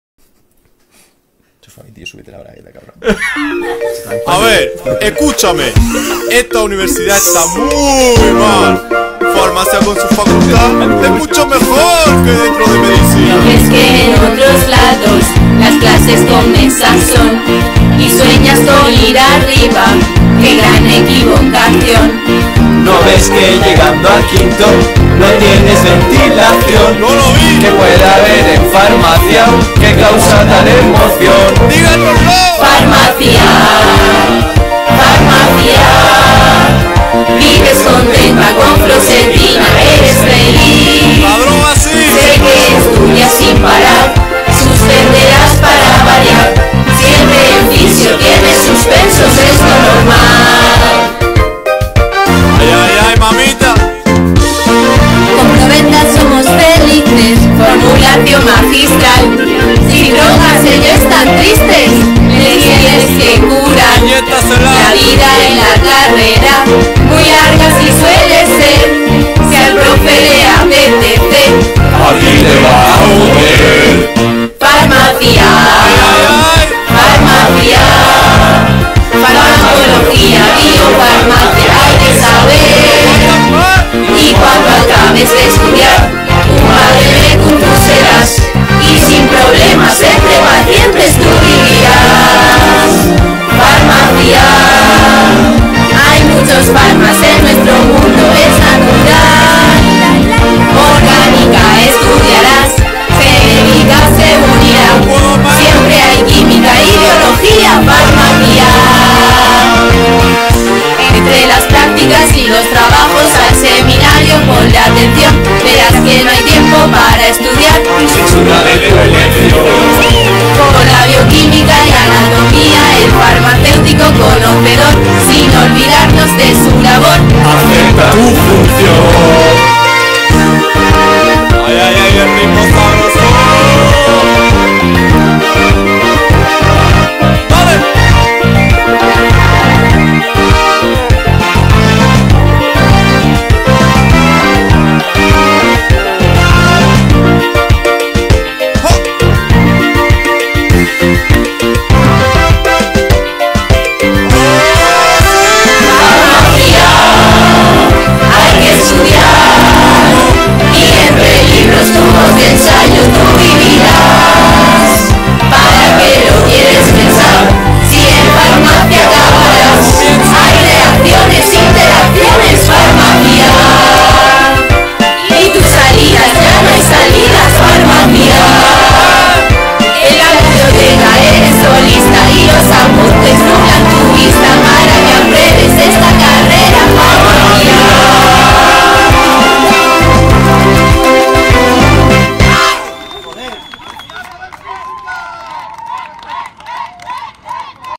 FARMACIA | Promoción 2012-2017 | Universidad de Murcia
Video promocional de Grado en Farmacia, interpretado por la promoción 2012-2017.

Universidad de Murcia.